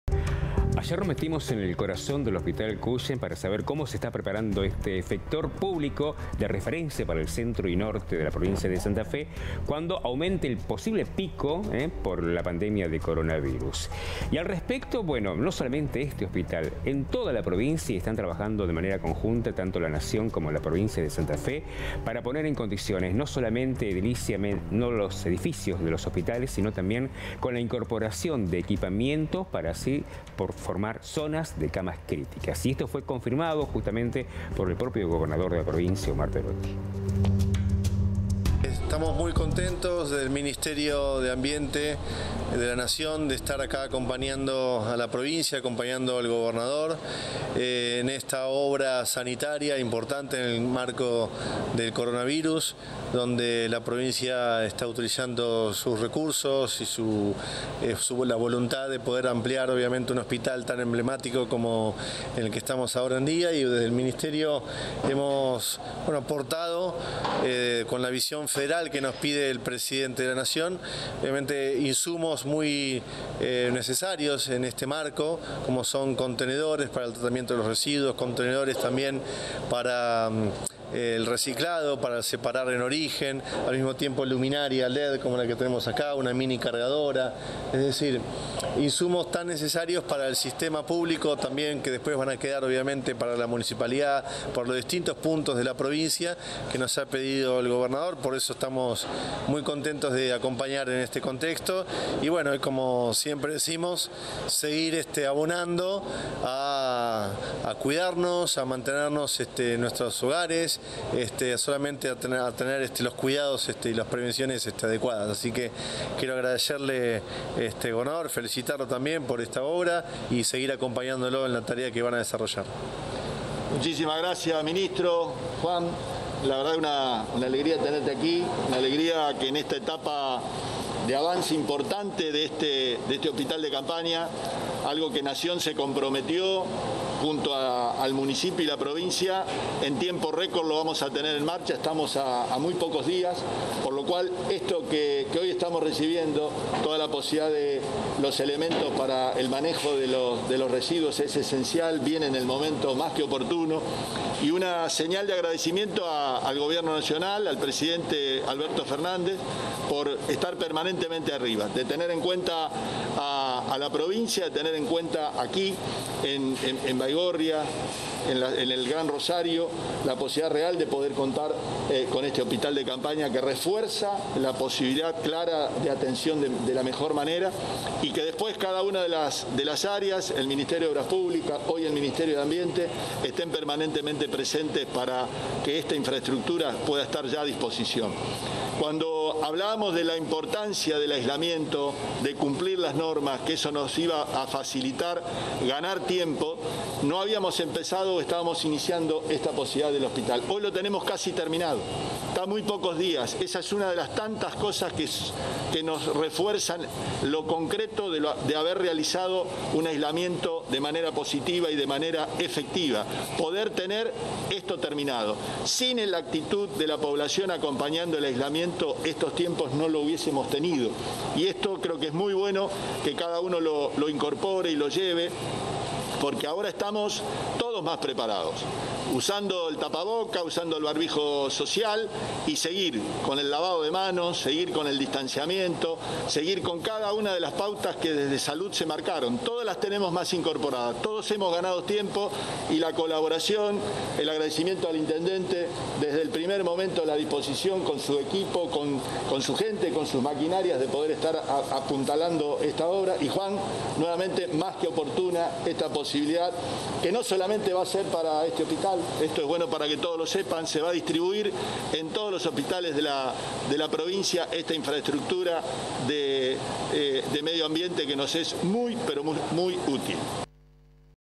Efectores públicos suman equipamiento en el marco de la emergencia sanitaria
El Ministerio de Ambiente y Desarrollo Sostenible de Nación hizo entrega de equipos para la separación de residuos, artefactos LED y una minicargadora.